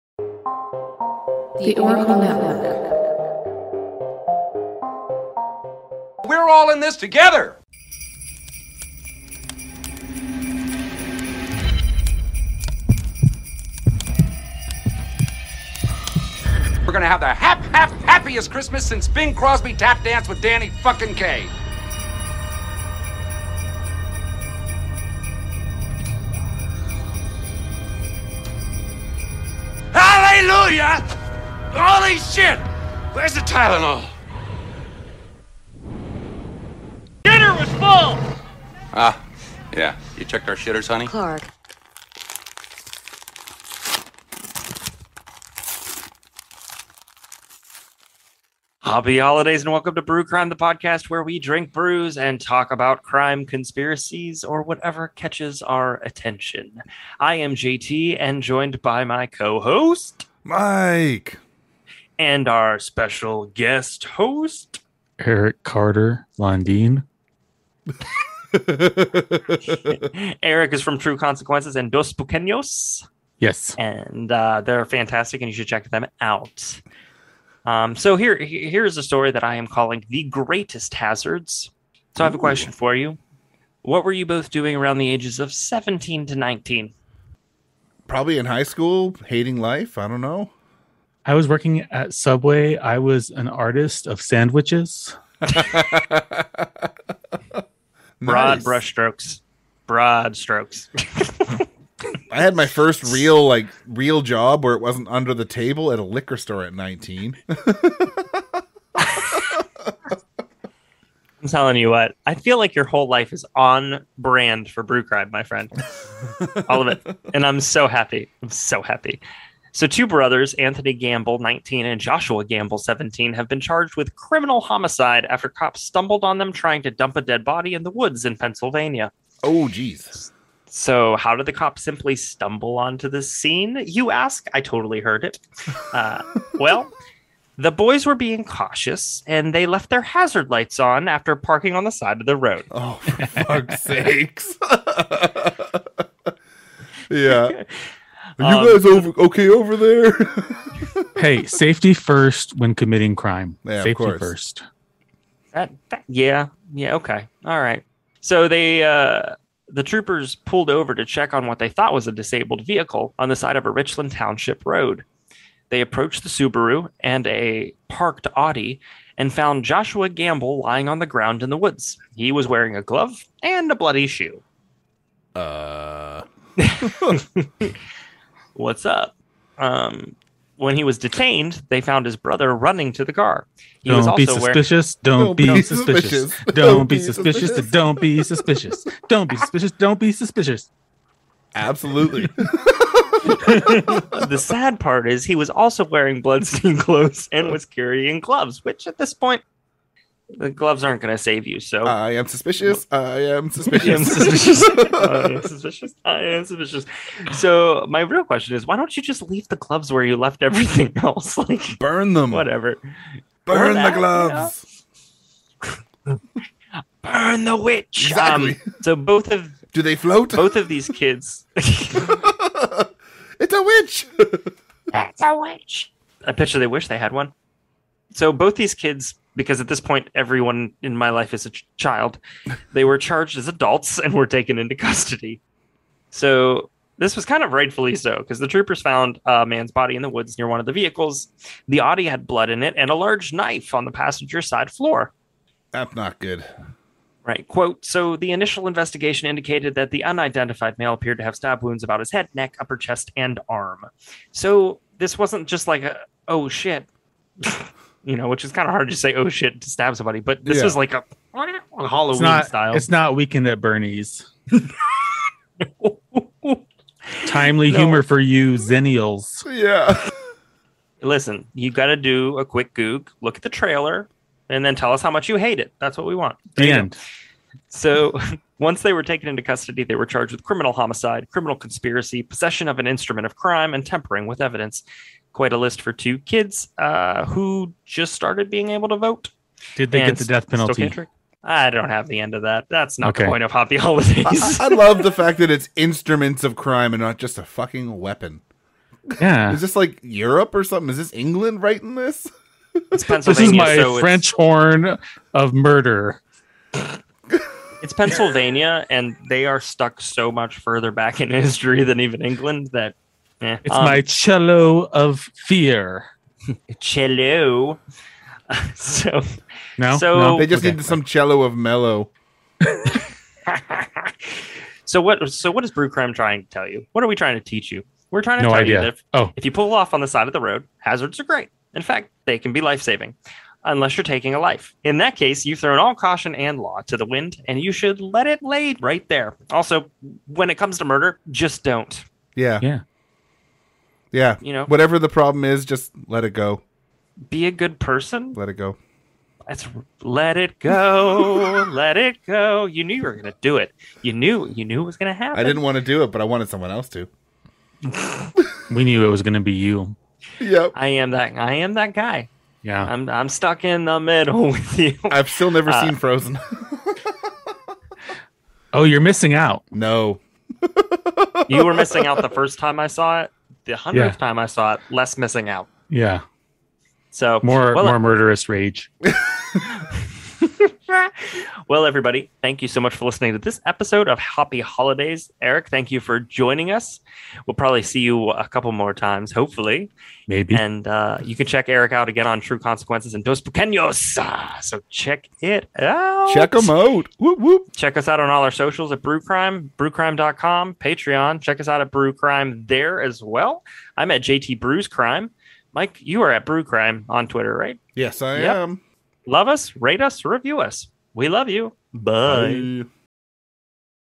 HH 25 - The Greatest Hazards
Welcome to Brew Crime Podcasts Hoppy Holiday series!  December will see daily short episode for the entire month of December. On this episode JT and Mike are joined by Eric Carter Landin of True Consequences Podcast and Dos Spookqueños.

Story – JT
The Greatest Hazards